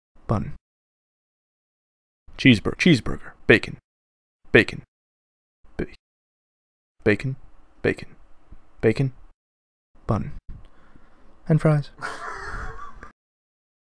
Buy My Buns
Quick edit of a burger assembly that I used to help a friend's college project